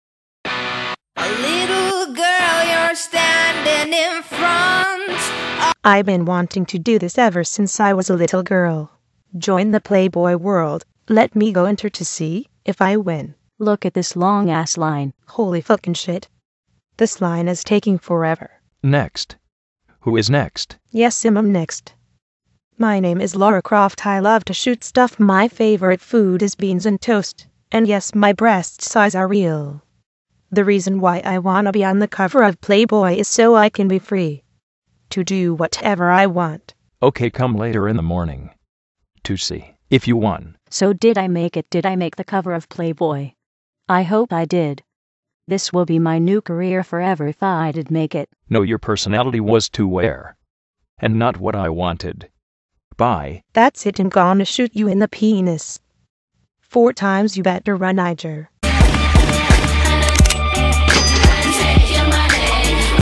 The Lara croft adventure (LARA JOINS PLAYBOY)
as a child lara has dreamed of this dream to join PLAYBOY will she make it? wacth